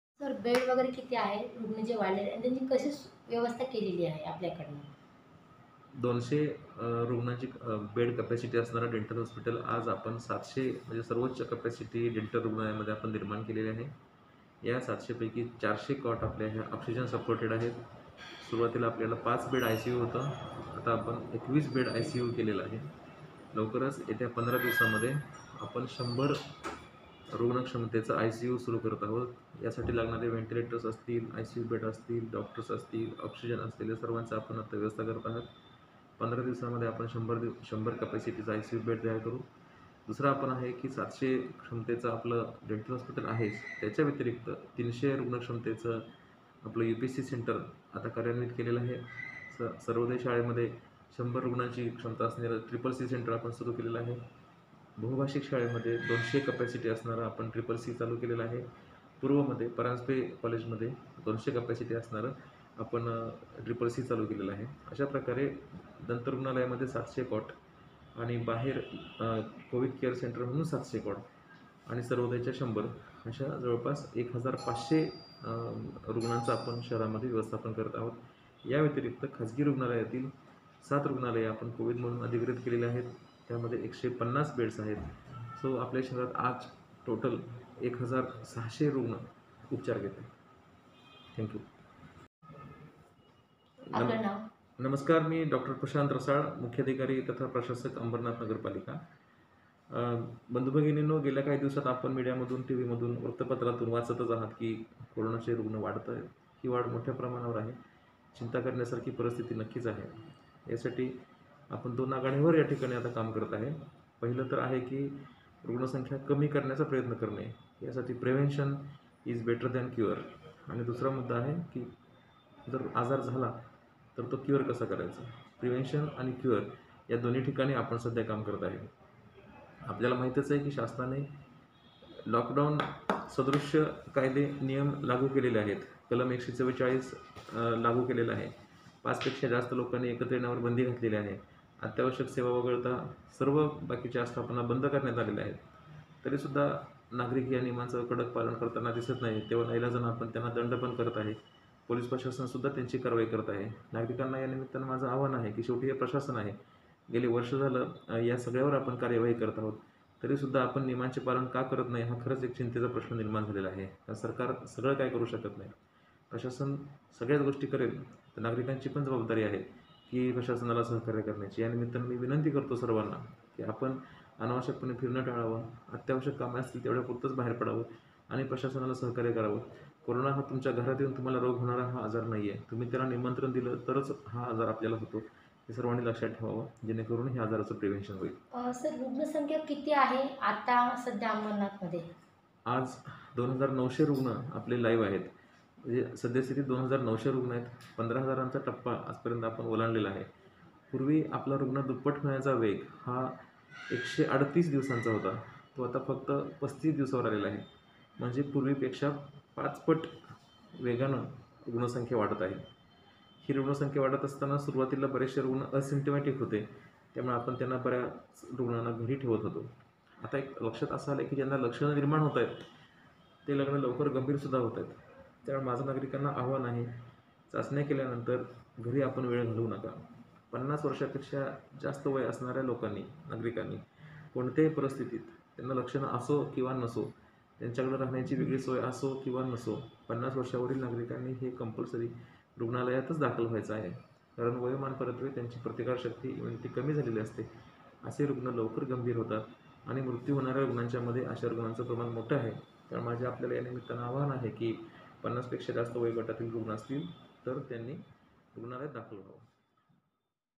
नागरिकांनी कोविड नियमांचं पालन करावं : प्रशांत रसाळ
मुंबई :  राज्यात कोविडचे रुग्ण हे वाढतच आहेत.  ही बाब चिंतेची आहे.   आपल्याला कोविड बाबत काही लक्षणे आढळल्यास प्रशासनाला कळवणे. असं आवाहन अंबरनाथ नगरपरिषदेचे मुख्य अधिकारी प्रशांत रसाळ यांनी केलं. राज्यात
कलम 144 लागू केलेला आहे पाच पेक्षा जास्त लोक एकत्र येऊ शकत नाही पाच पेक्षा जास्त असल्यास त्यांच्यावर गुन्हा दाखल होतो प्रशासन एवढेच सर्वकाही करत आहे तरीसुद्धा नागरिक त्याचं गांभीर्य लक्षात येत नाही आहे शेवटी प्रशासन आहे मी सर्व नागरिकांना विनंती करतो विनाकारण फिरणं टाळावं.

 अत्यावश्यक काम असेल तरच घराच्या बाहेर निघावे आणि प्रशासनाला सहकार्य करावे कोरोना हा हा आजार आहे जो तुम्हाला तुमच्या घरी राहून होत नाही तुम्ही त्याला आमंत्रण दिल्यावरच हा आजार होतो असे अंबरनाथ नगरपरिषदेचे मुख्य अधिकारी प्रशांत रसाळ यांनी म्हटले आहे